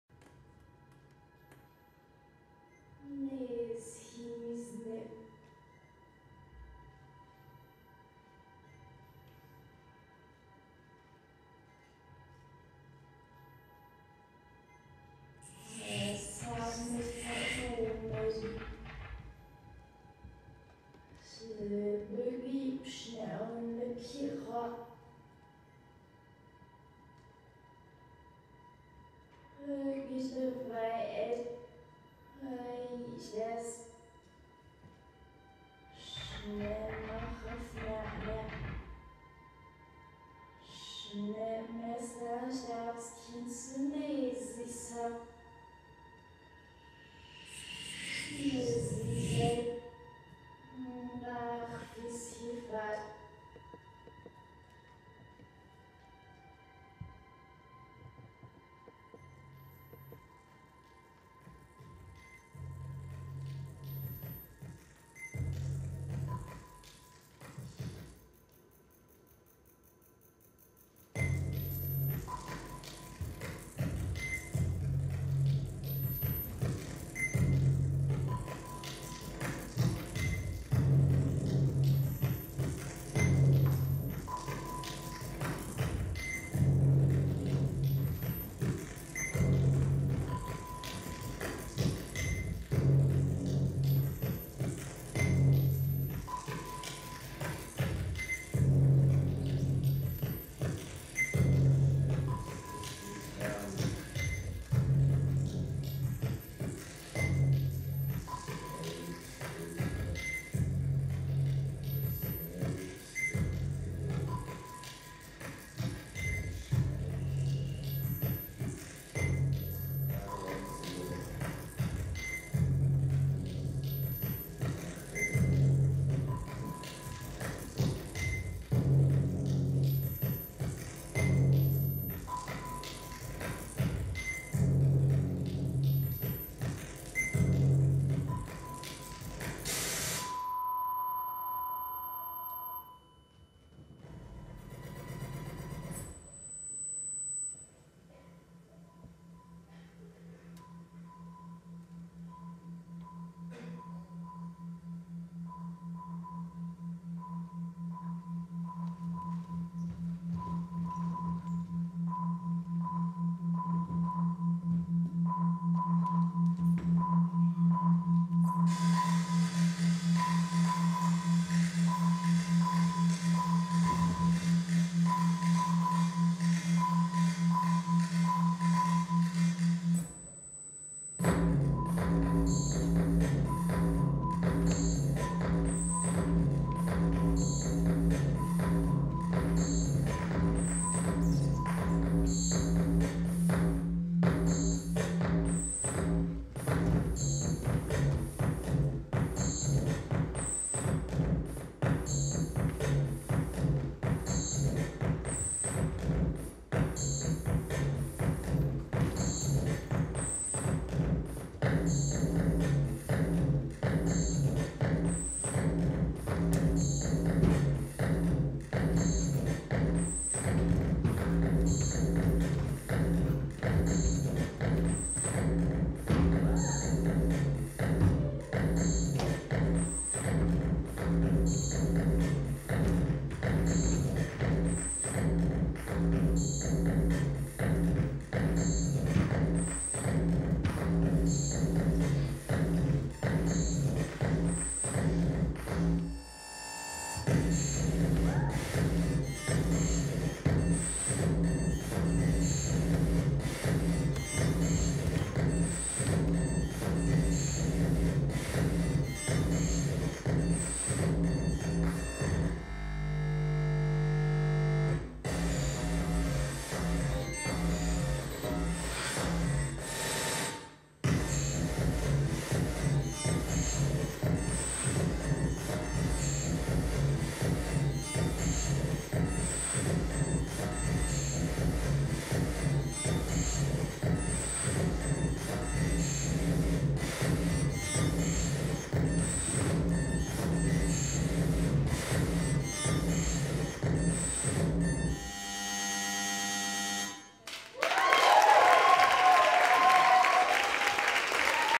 Contemporary Dance Piece: Workshop Choreography
A piece I performed in my first semester of 2nd Year at London Contemporary Dance School.

Piece:
Choreographer: Ricardo Dasilva